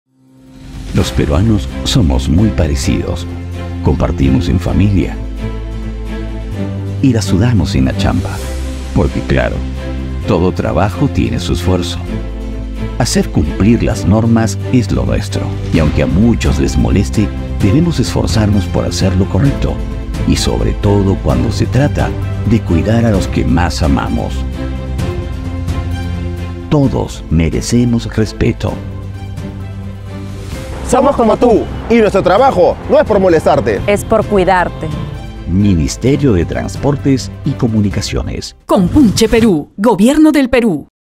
MTC lanza la campaña "No es por molestarte, es por cuidarte"
🔴EnVivo | El ministro de Transportes y Comunicaciones, Raúl Pérez Reyes, junto a los titulares de la Superintendencia de Transporte Terrestre de Personas, Carga y Mercancías u la Autoridad de Transporte Urbano, lanzan la campaña 🖐 "No es por molestarte, es por cuidarte", que promueve el respeto al trabajo de sus inspectores y fiscalizadores.